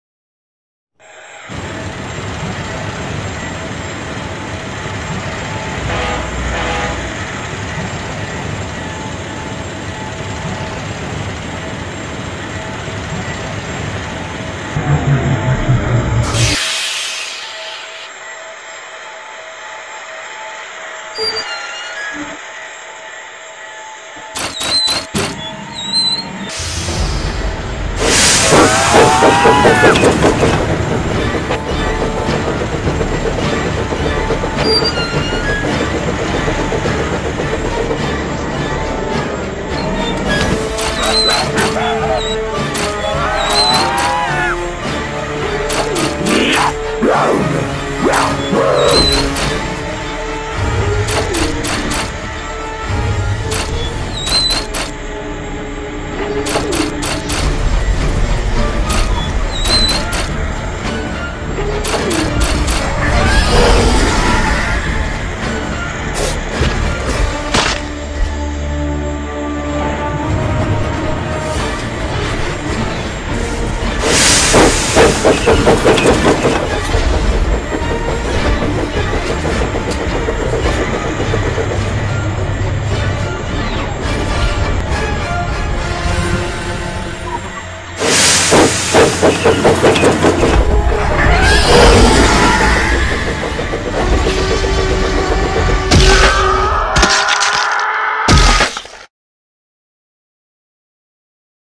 Sodor Fallout: The combat test (2/3) (read the description)
so here is the second part that i had made so far, and i apologize with some of the lazy animations along with Diesel 10's claw since i was kinda busy attended a few things personal. but some of you are welcome if you want to either watch or enjoy the video.

Some of the scenes were inspired by Percy and the beast productions, Ashton M 1920, and BZRailways

all content & sfx belong to their respectful owners.

sodor fallout belongs to TidmouthThunderbolt.